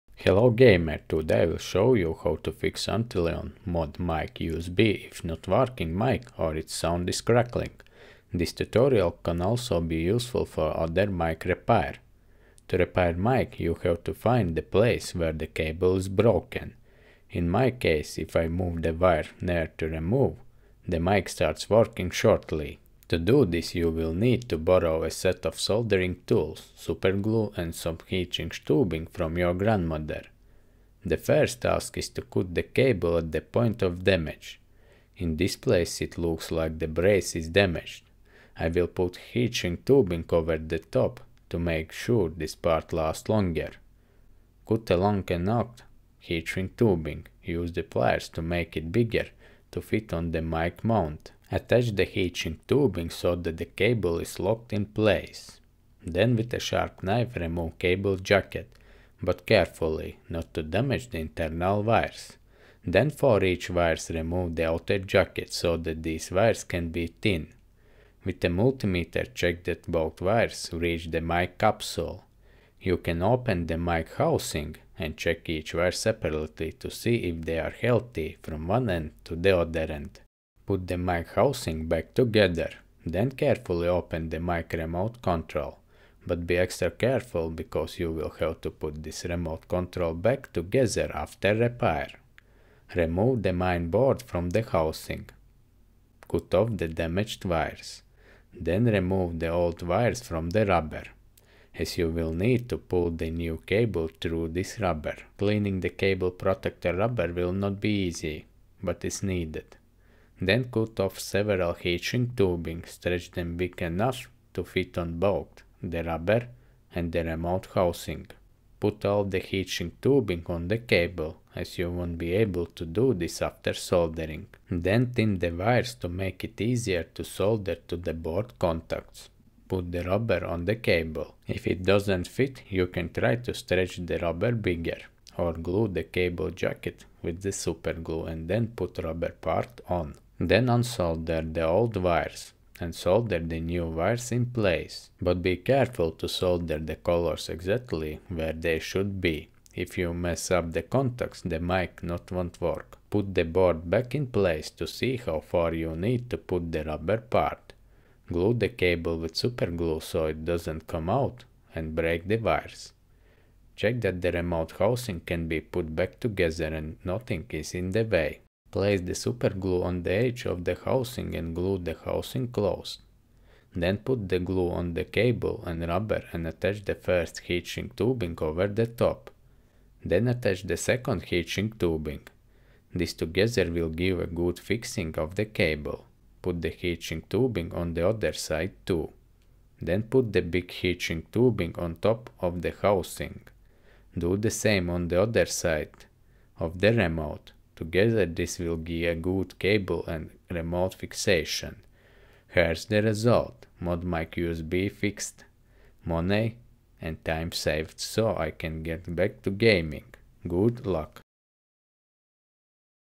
How to Repair ModMic When Mic Not Working
How to Repair broken Antlion ModMic USB When Mic Not Working. In this video, I'll show you how to troubleshoot and fixing your ModMic usb or 3.5mm version when it's not working properly. Whether it's not picking up sound, having connection issues, or simply not functioning, I'll guide you through a step-by-step process to get it back in working order. Follow along with this easy DIY tutorial to fix your ModMic and save yourself from buying a new one.

Hазвание видео: Как починить Antlion ModMic USB, если микрофон не работает
🇱🇻 Video nosaukums: Kā salabot Antlion ModMic USB, ja mikrofons nedarbojas
🇩🇪 Video Titel: Wie man den Antlion ModMic USB repariert, wenn das Mikrofon nicht funktioniert